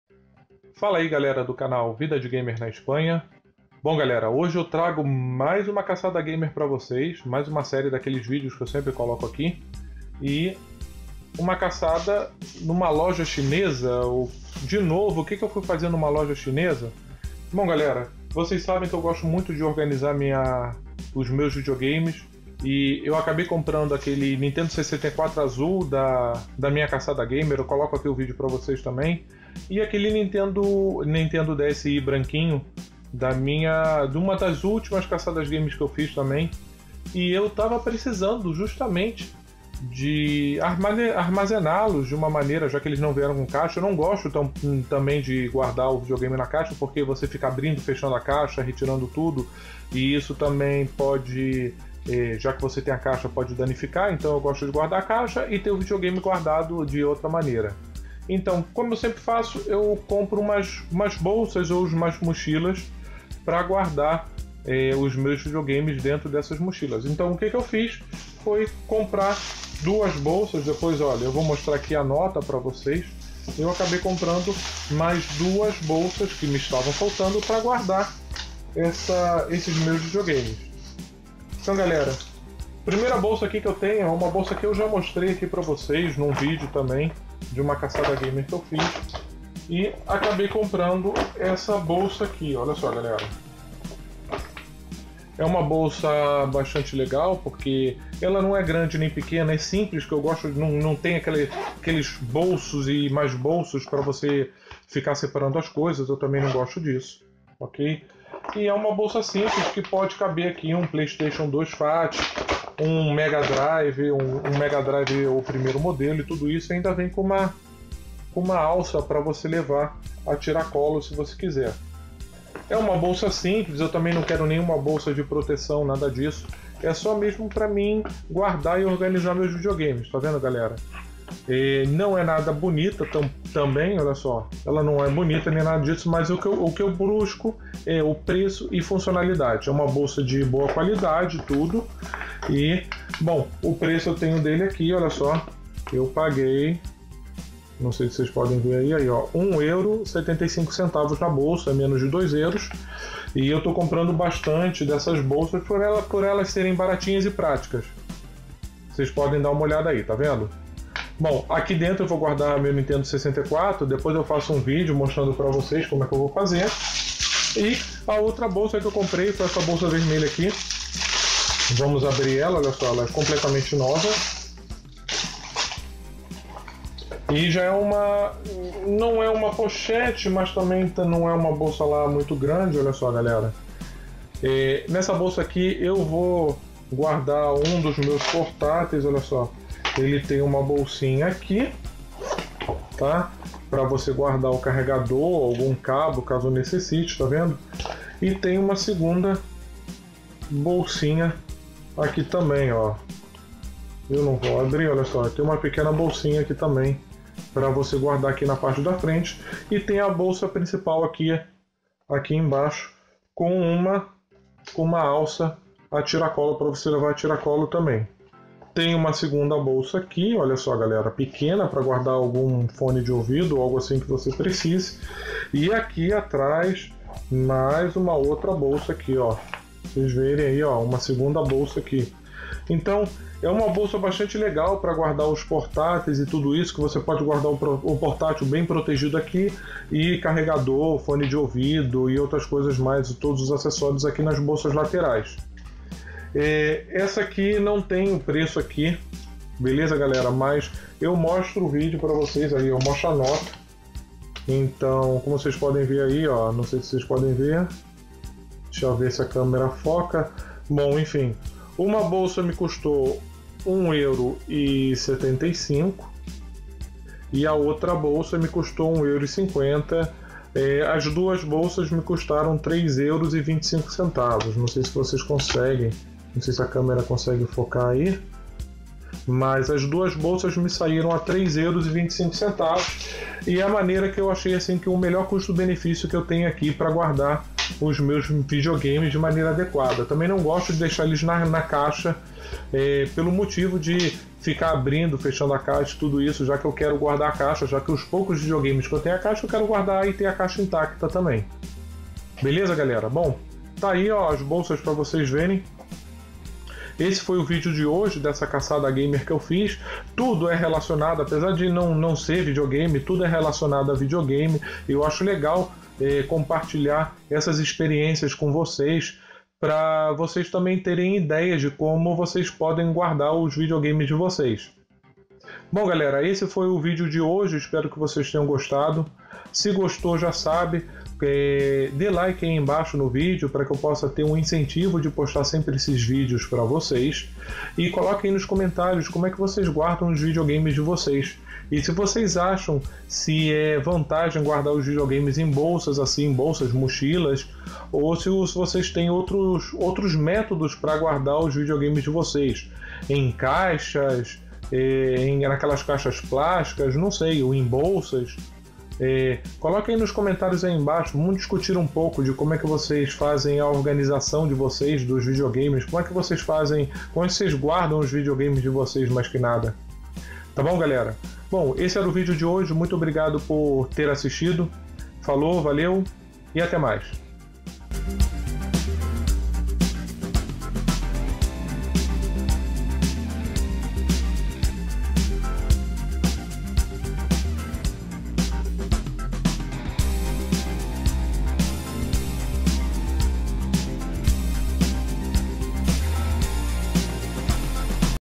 Caçada Gamer 24 - Tudo a Ver com VideoGames - Vida de Gamer na Espanha
Fala galera !!!

Hoje mais um vídeo de caçada, porêm, uma caçada um pouco diferente mas tudo a ver com videogames, claro!